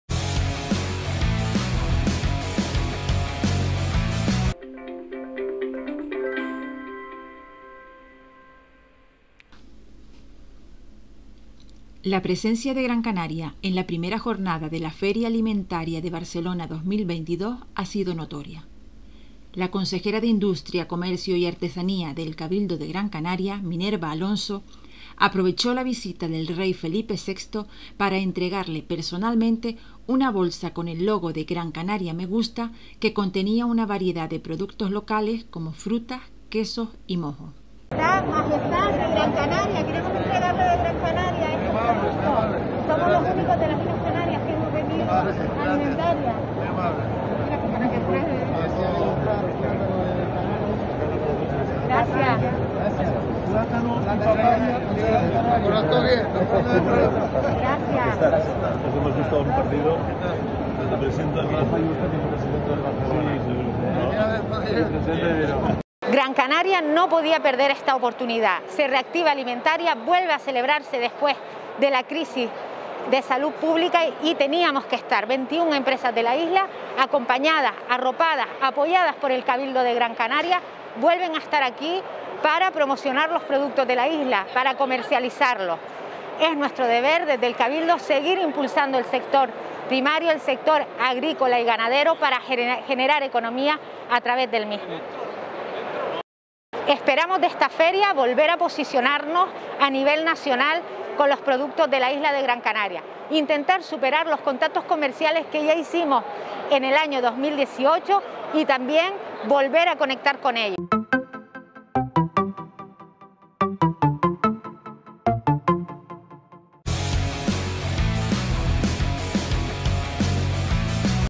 Empresas de Gran Canaria acuden a la Feria Alimentaria de Barcelona
Las empresas de GranCanaria acuden a la Feria Alimentaria de Barcelona 2022 para abrirse al mercado nacional e internacional. La consejera de Industria, Comercio y Artesanía del CabildodeGranCanaria, MinervaAlonso, tuvo la oportunidad de entregarle en persona al ReyFelipe una muestra de productos locales: frutas, quesos y mojos

@AlLimite_noticias  nuestro nuevo canal de YouTube para noticias nacionales e internacionales